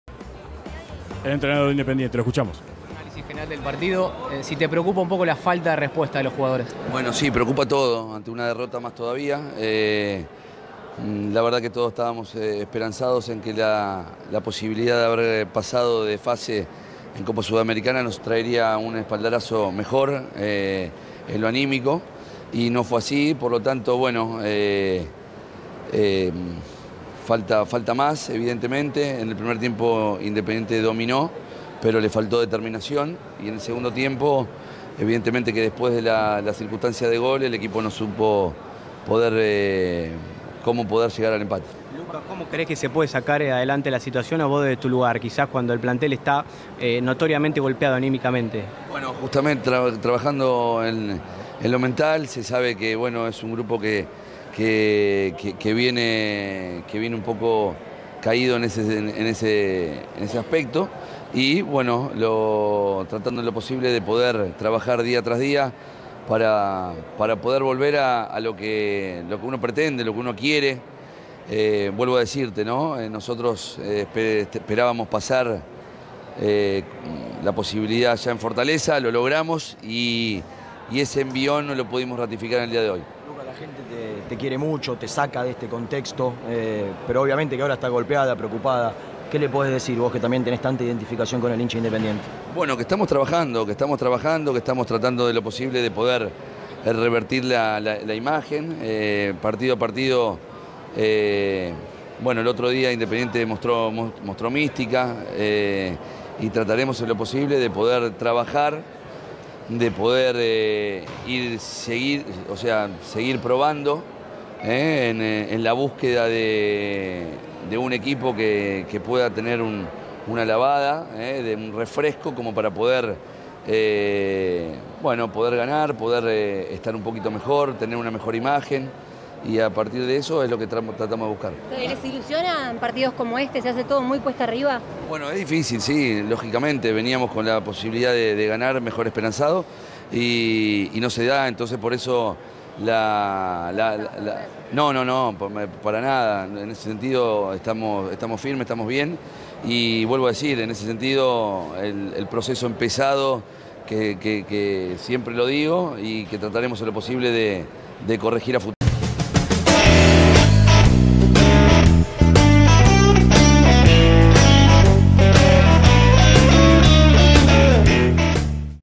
Conferencia de prensa de Lucas Pusineri
TNTSports | El DT de Independiente conversó con los medios tras la derrota conseguida ante Huracán por el cierre de la Fecha 22.